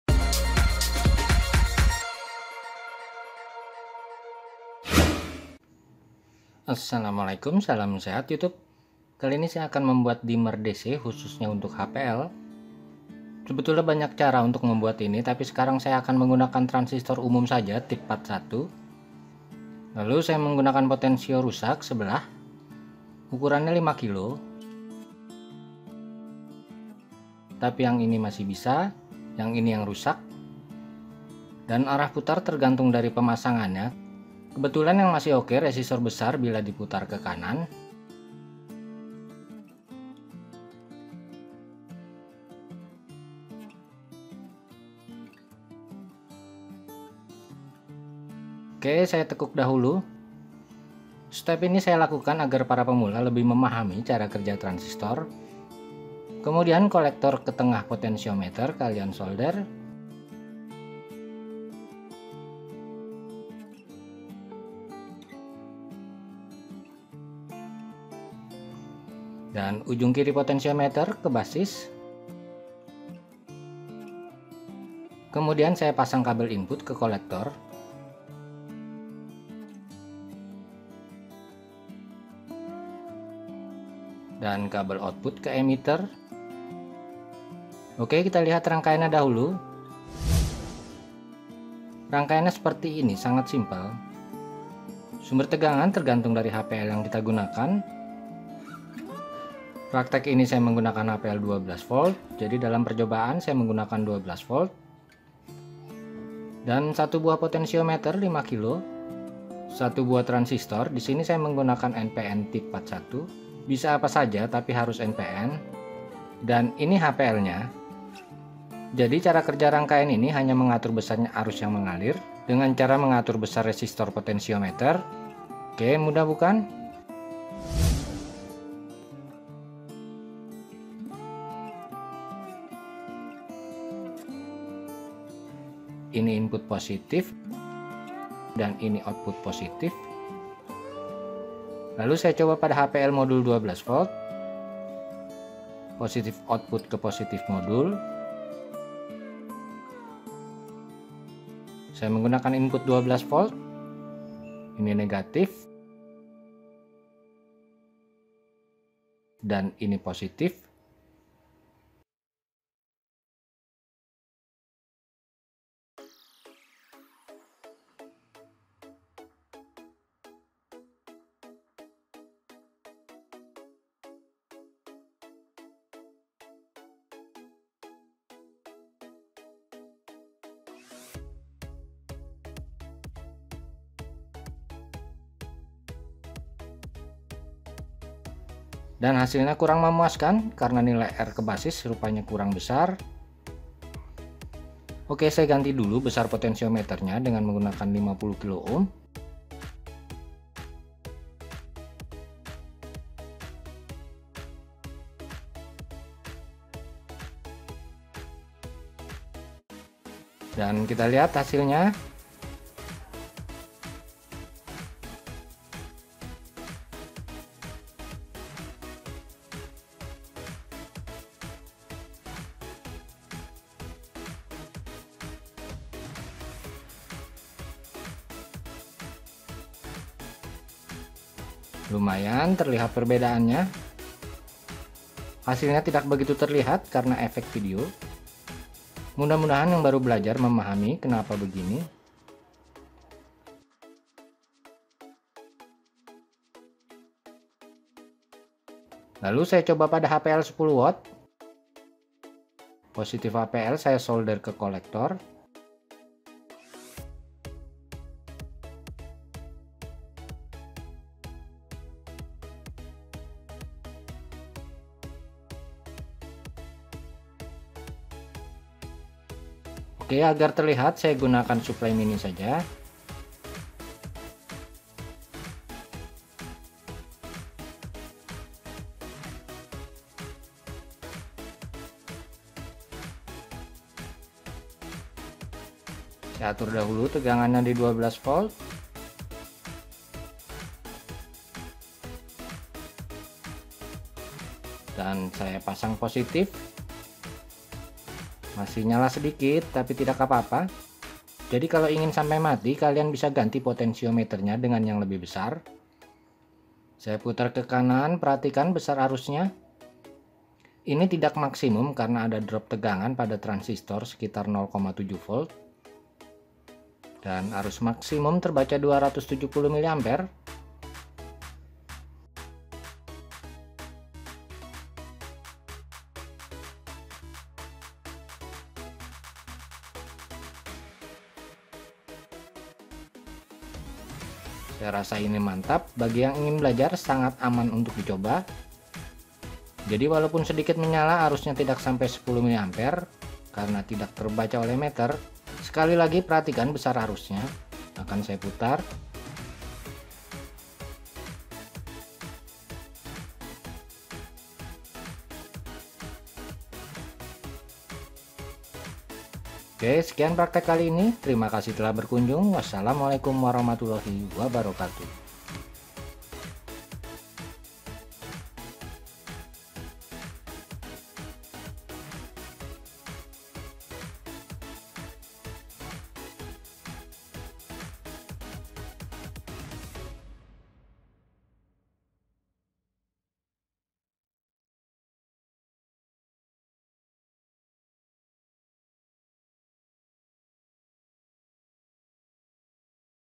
Dimmer DC Led HPL 10W termurah ✔ cara membuat dimmer dc 12 volt
Video ini menjelaskan cara membuat dimmer dc khususnya untuk HPL.

00:00 Radal05
00:17 Bahan yang digunakan
00:56 Assembly